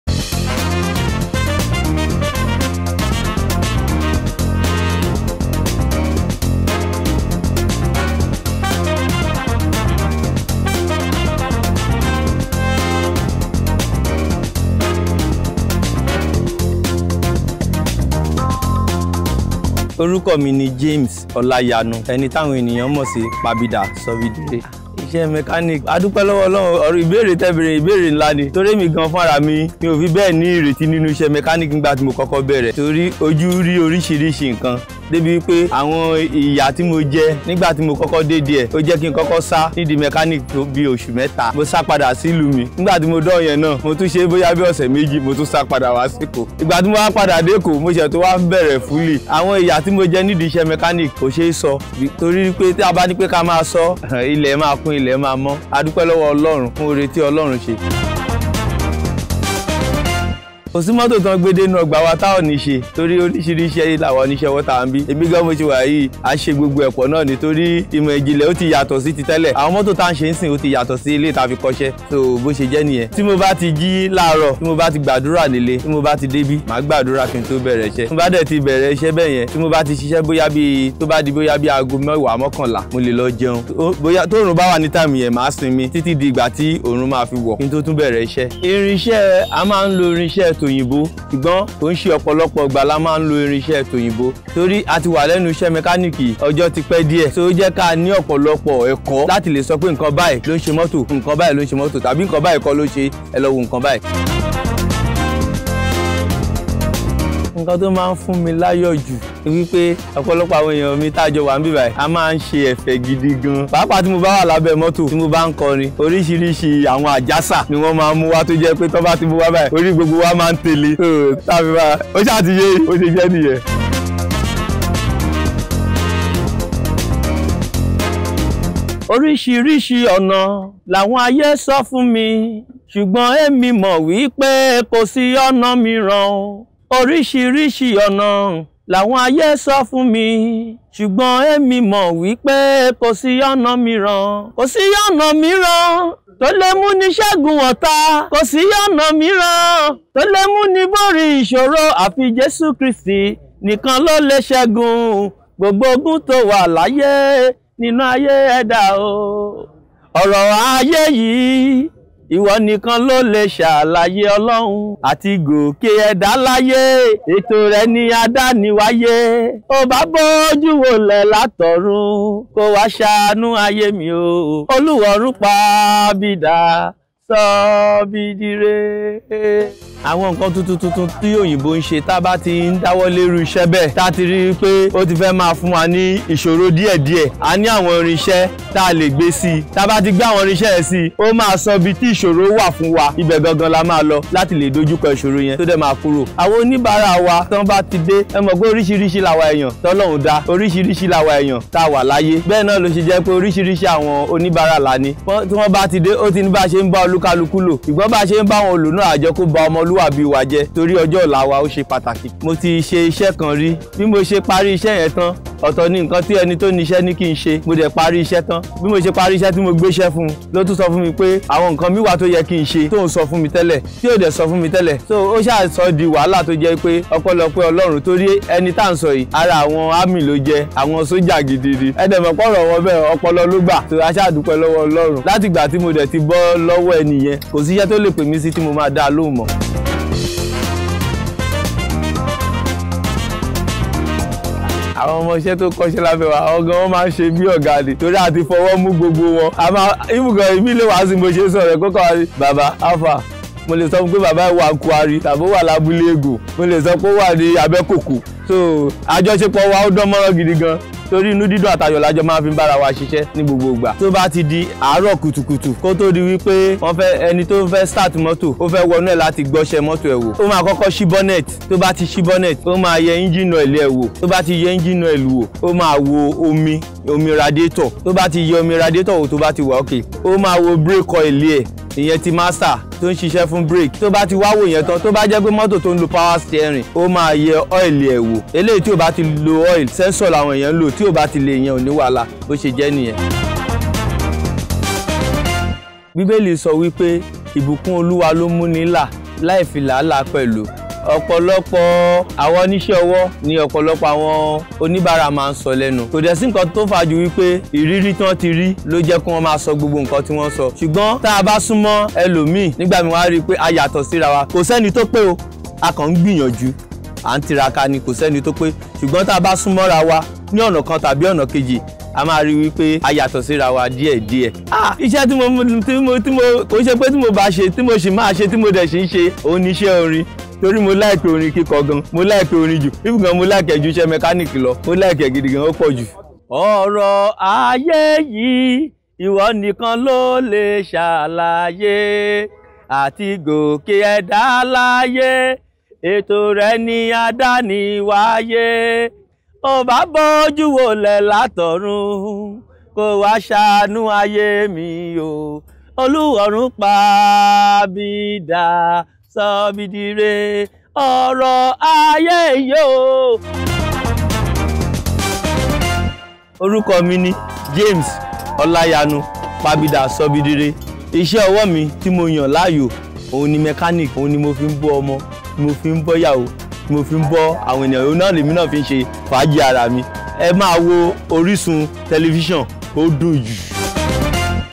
Ise Owo Pelu James Olayanju | OrisunTV
Master of Machines: Celebrating the Handiwork of James Olayanju! 🙌💪

Meet James Olayanju, the unsung hero of the automotive world. With his skilled hands and boundless passion, he breathes life into engines and keeps our wheels turning. 🚗✨

Every bolt, every wrench turn, and every engine roar is a testament to his dedication. James is not just a mechanic; he's a magician in overalls, making vehicles purr like kittens. 🛠️🐱

Let's show some love for this hardworking mechanic extraordinaire. Drop a comment if you've ever had James work his magic on your car! 🗣️🚘